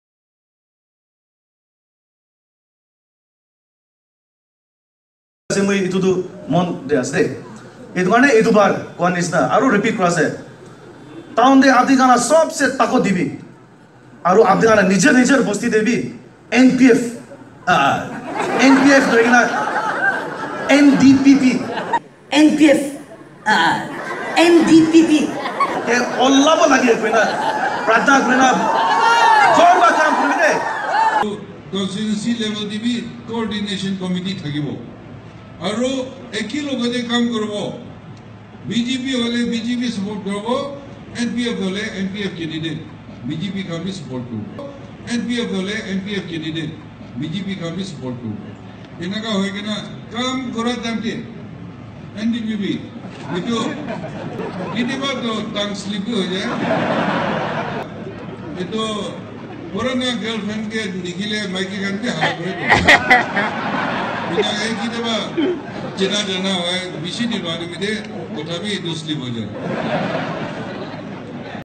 Sense of humour - NPF AH... NDPP - Nagaland Election Campaign Speech bloopers (Funny Nagamese video)
NPF AH NDPP - Funny Nagaland Election 2018 Campaign Speech Bloopers
With frequent party changes, the leaders seems to be confused with the party names.
But they quickly covered up with their sense of humour :)

Just for laughs and no ill intended.

Funny Nagamese Video.

Mobile video footage from social media sources. Sorry if resolution is not up to mark.